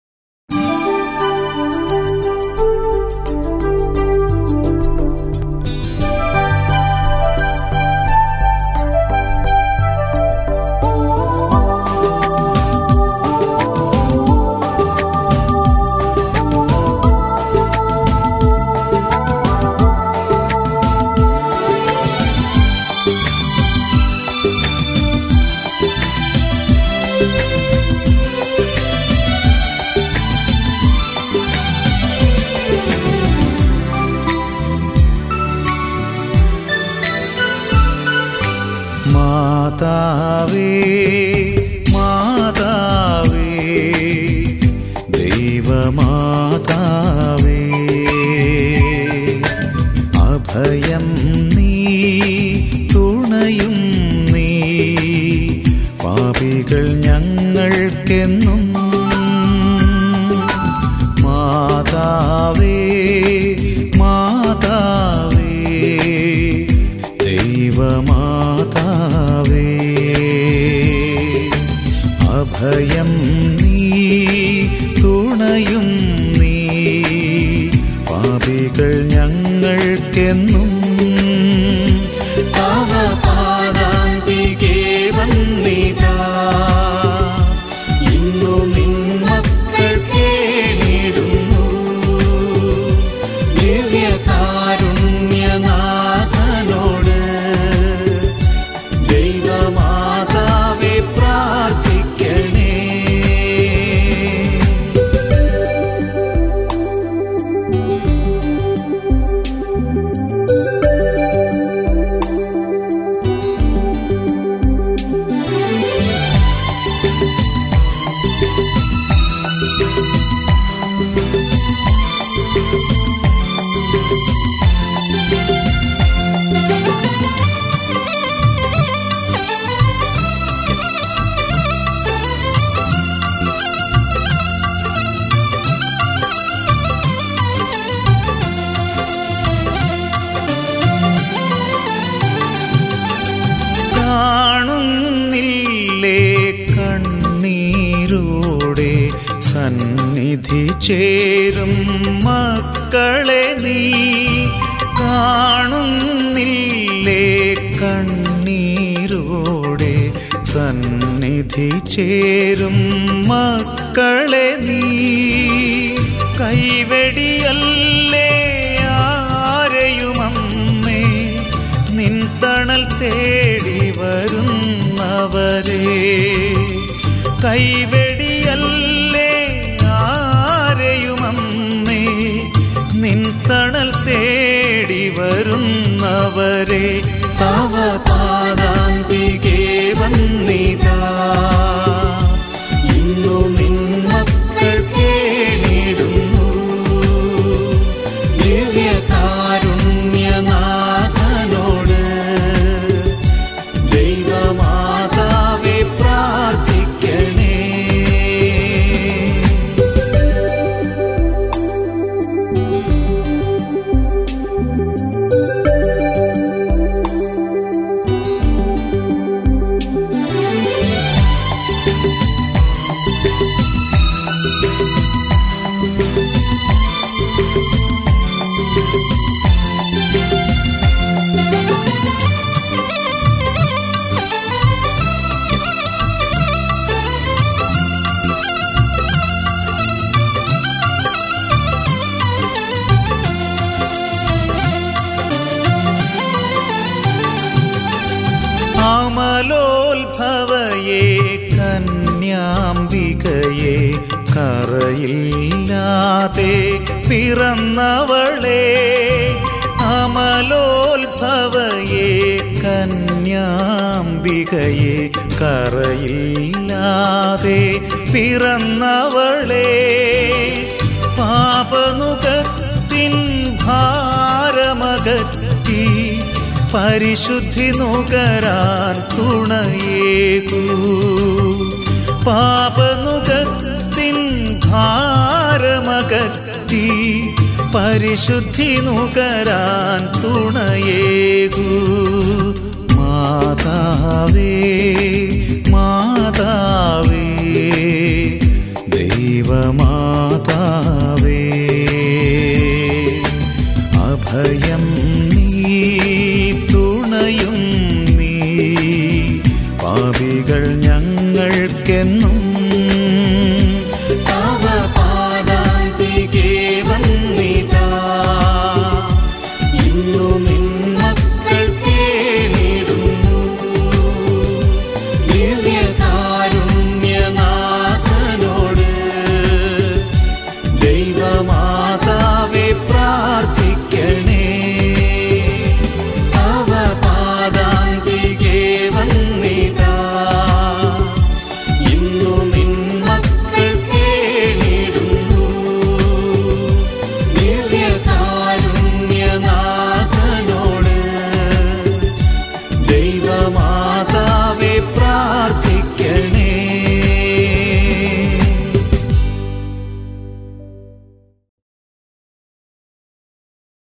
Chemperi Lourde Matha Forane Church
Visuals during 2020 October (10 Days Rosary of Blessed Virgin Mary)